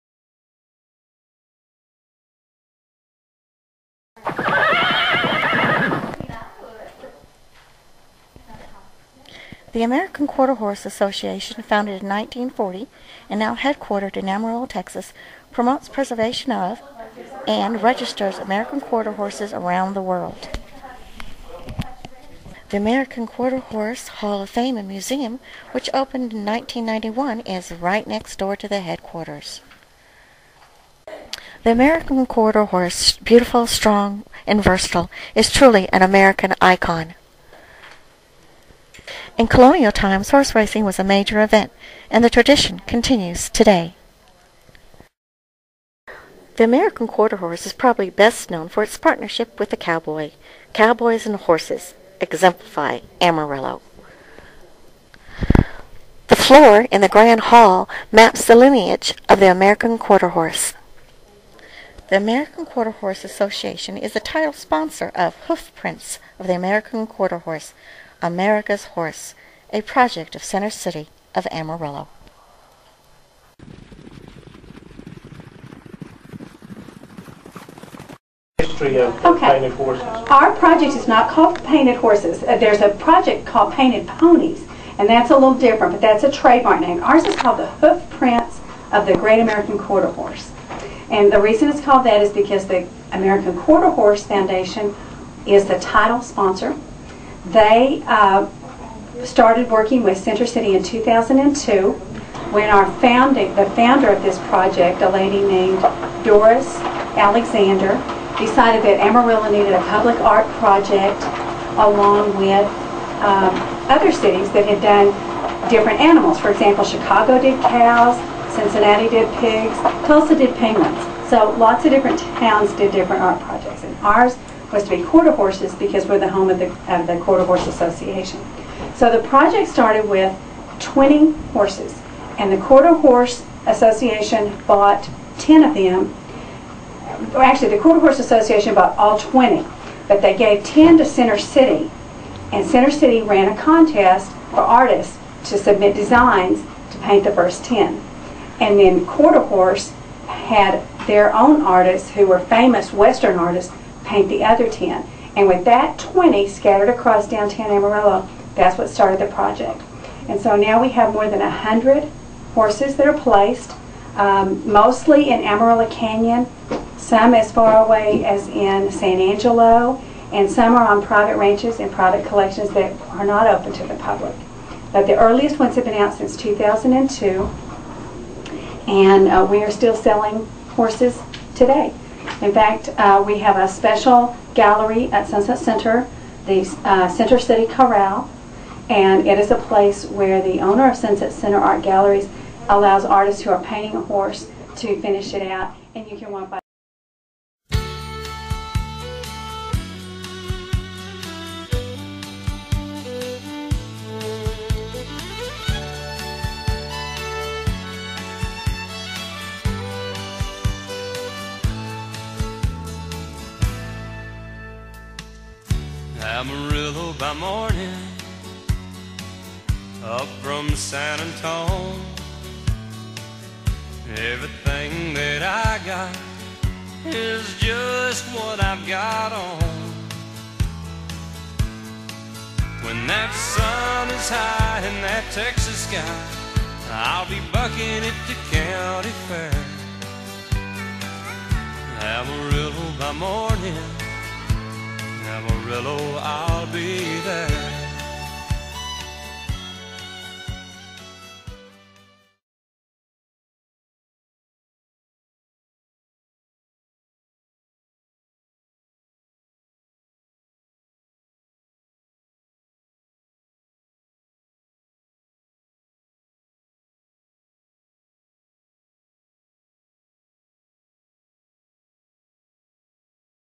Hoof Prints of Amarillo
A short video describing the origins of the painted horse statues located around Amarillo. Created as a project in a summer workshop.